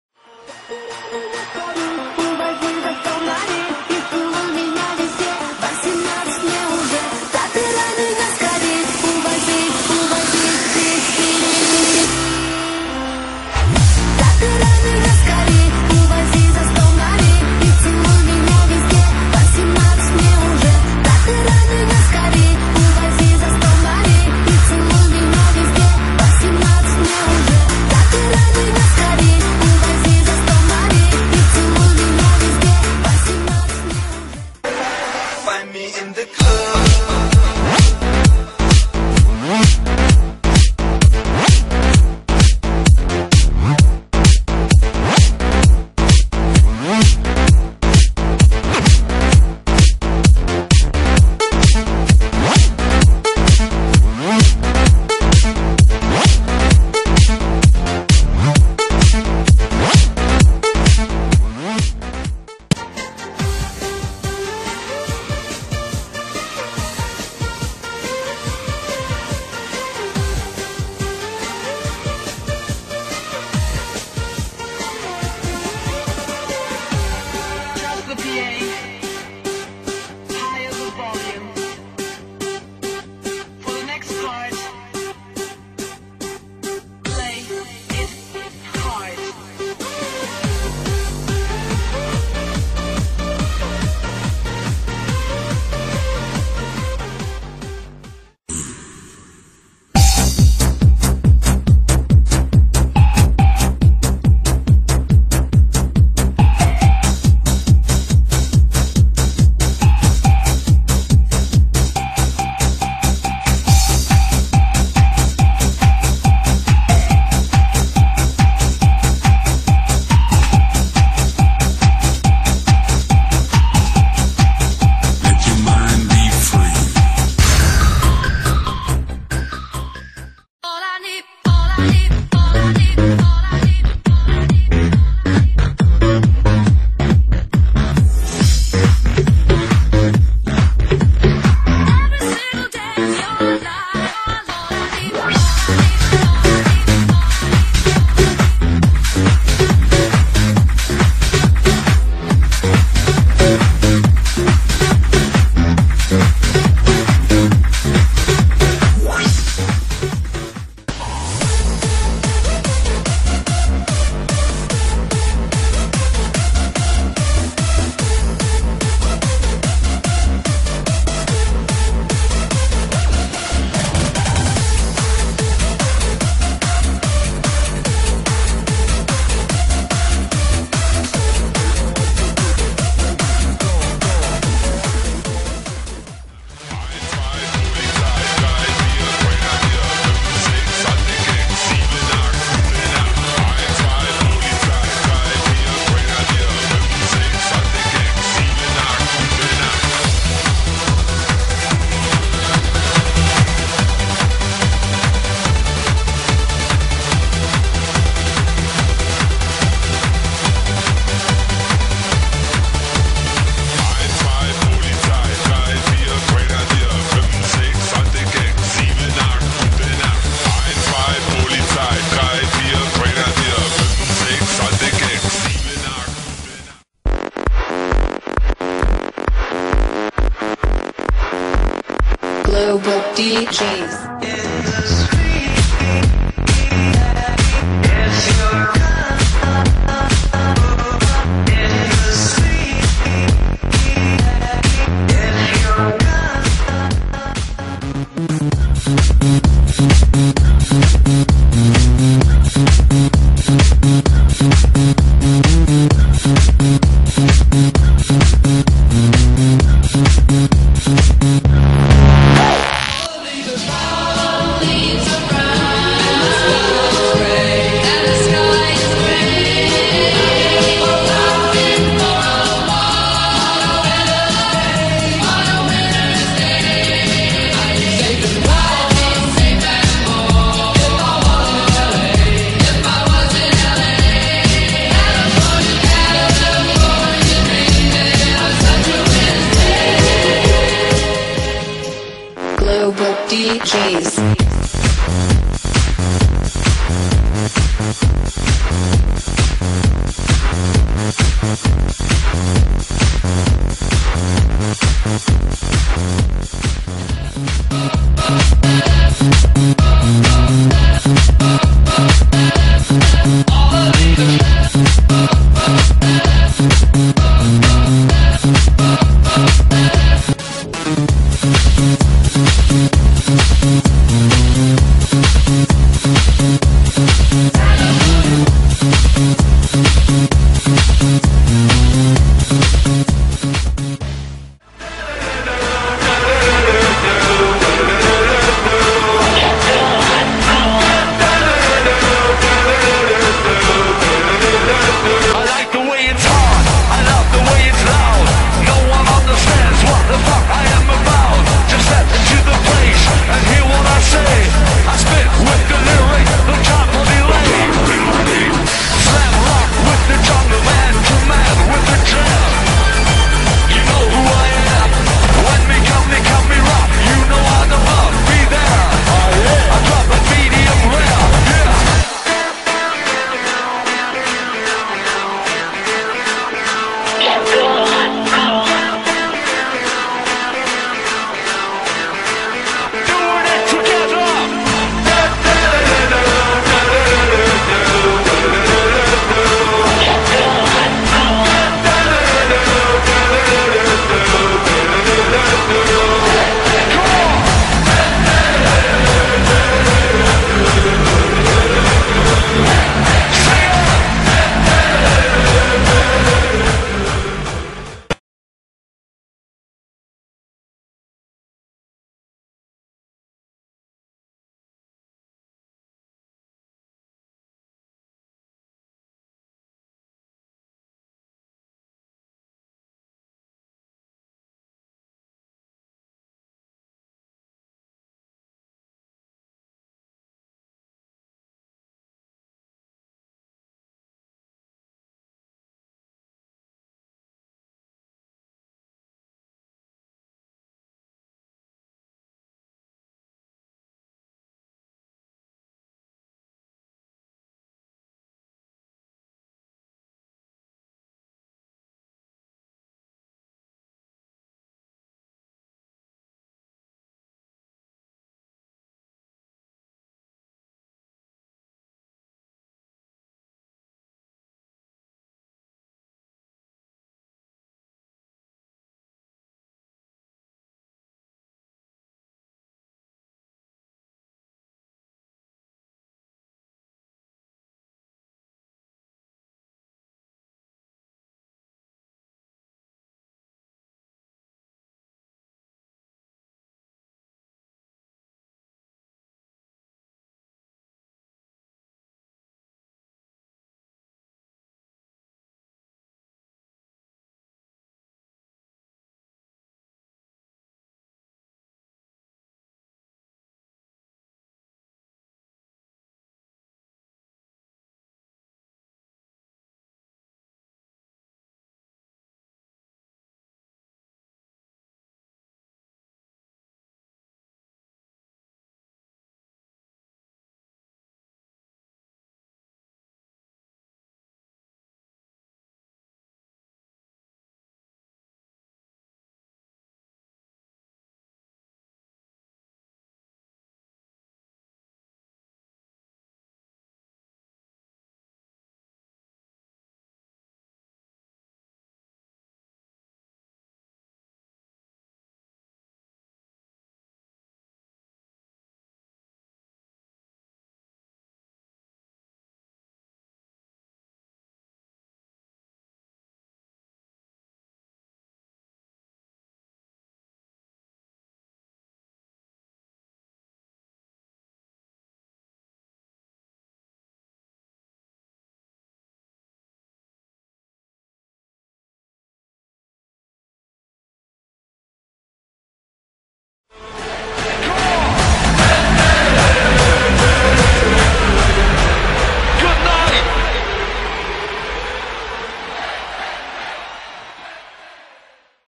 Techno 2009
Techno 2009. Where the Beats Come Alive. From Code Red, DJ Jean, DJ Quicksilver, MoDo, Global Deejays, Melbourne Shufflers, DJ Dean, DJ Antoine and Scooter